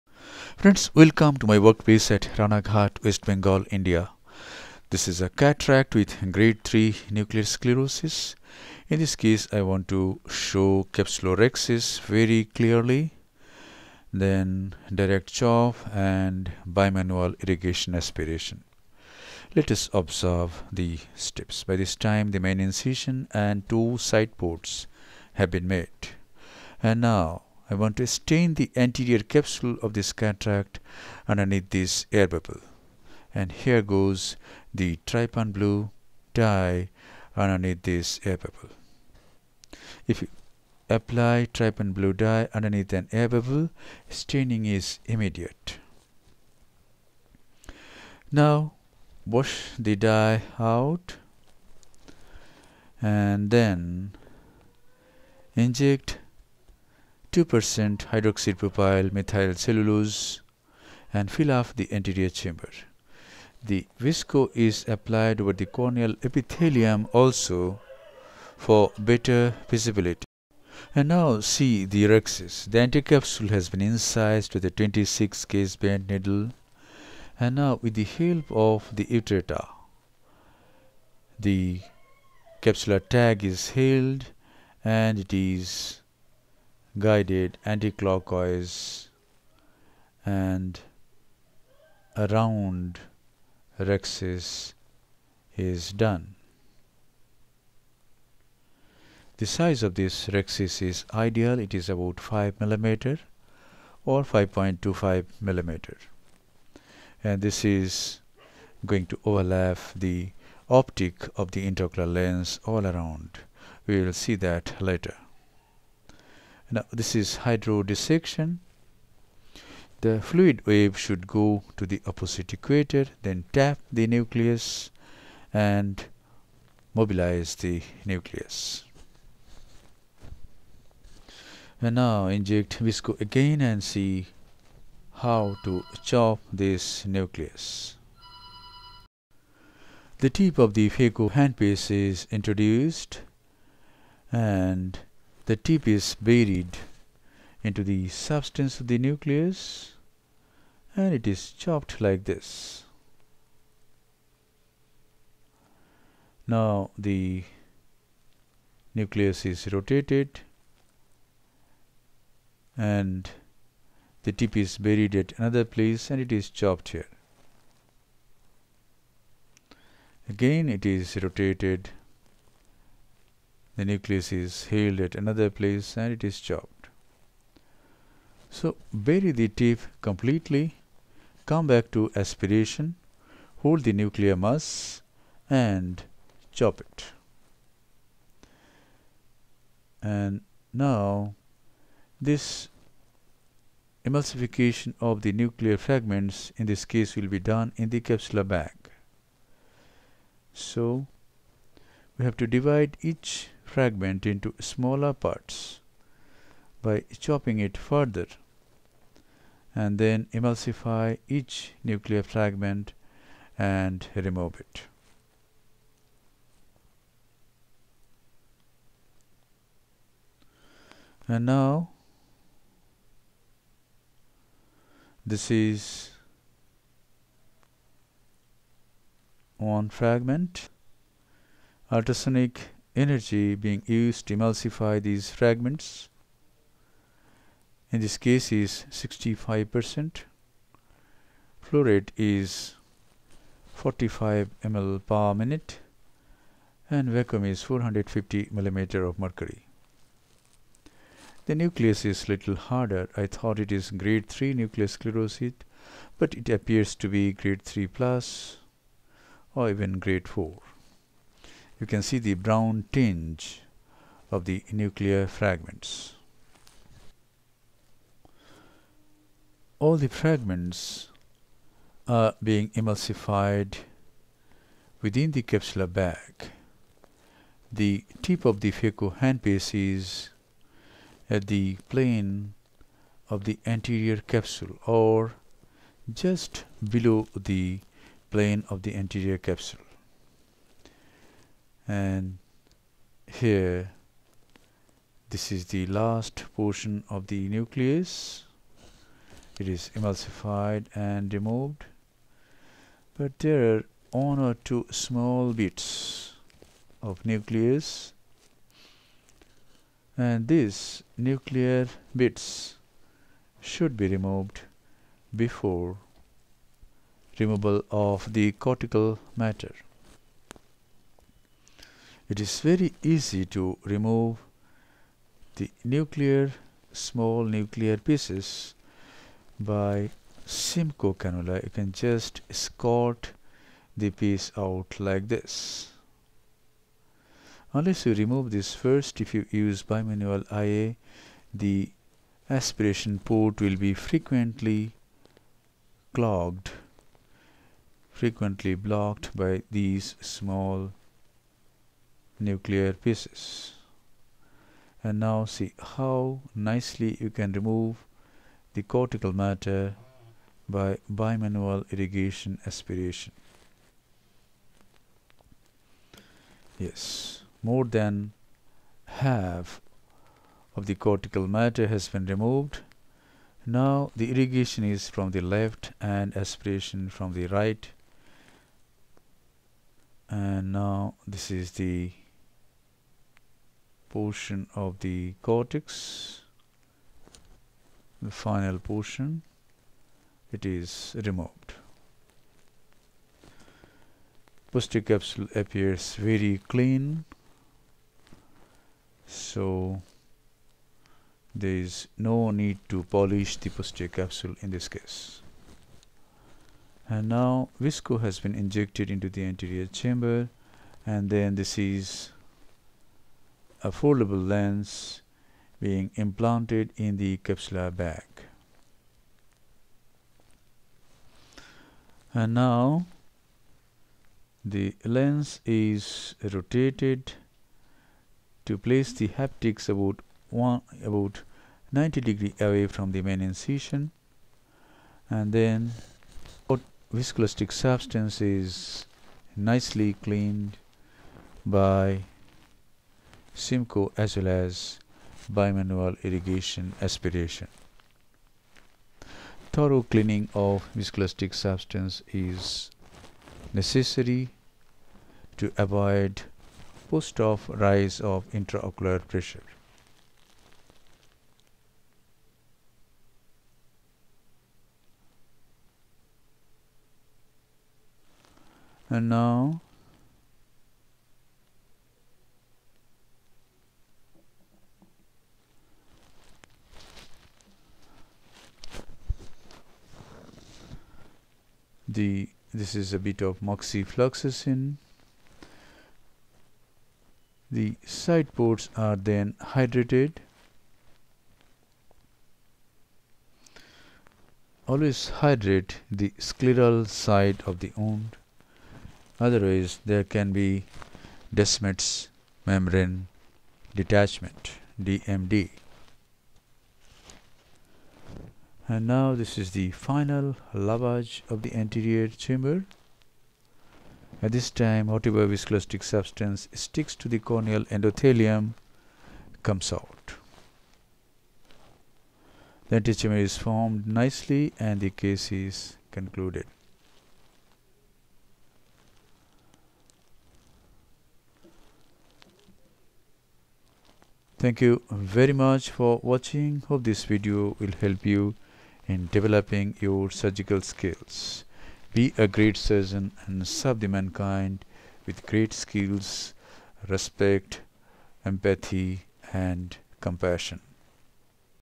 Emulsification of the nuclear fragments in the capsular bag : Pradip Mohanta 22/01/2019
To protect the corneal endothelium, this is the best way.